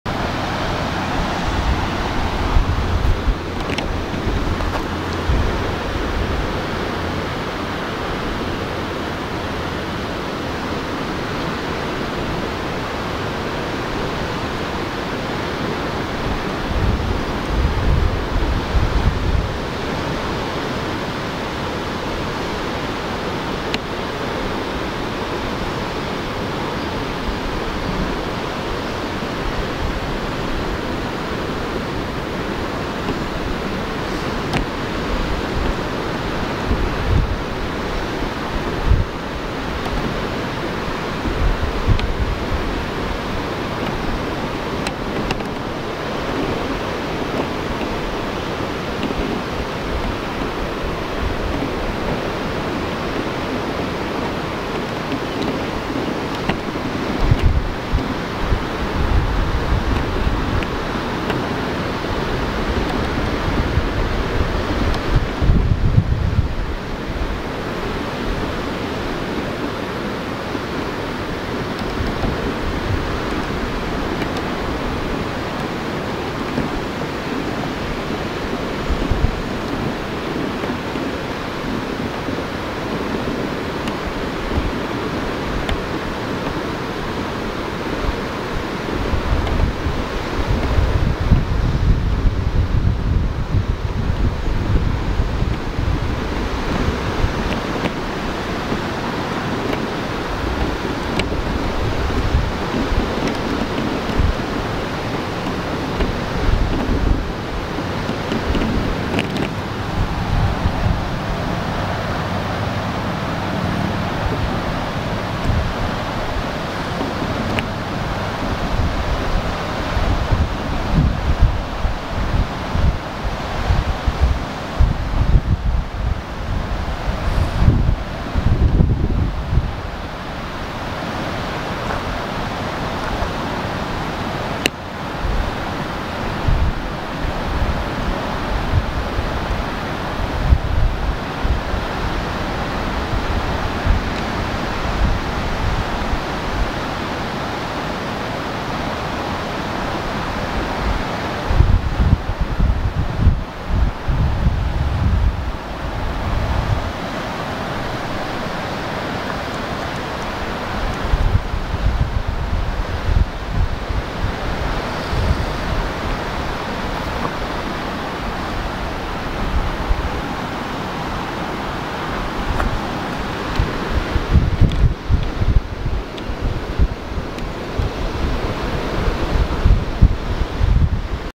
Oregon Coastline
Video of Oregon Coastline a little south of the town of Gold Beach, Oregon. This is close to the Natural Bridges and Arch Rock area.